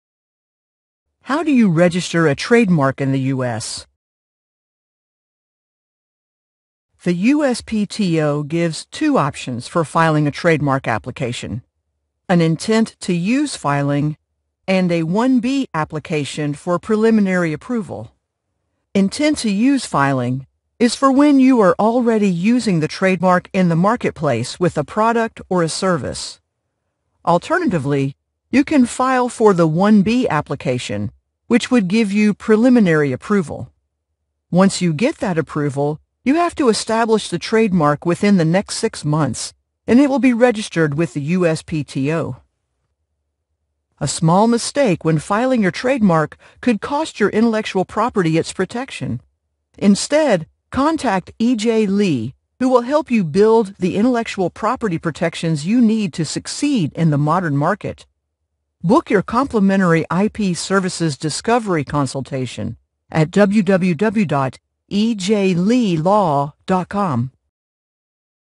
US Trademark And Copyright Attorney Explains How To Register A Trademark
Virtual Office Hours:
9AM - 5PM (Mon)
By Appointment (Tue - Fri)
United States

The USPTO gives two options for filing a trademark application: an intent to use filing and a 1B application for preliminary approval.
Intent to use filing is for when you are already using the trademark in the marketplace with a product or a service.
Alternatively, you can file for the 1B application, which would give you preliminary approval.
Once you get that approval, you have to establish the trademark within the next six months, and it will be registered with the USPTO.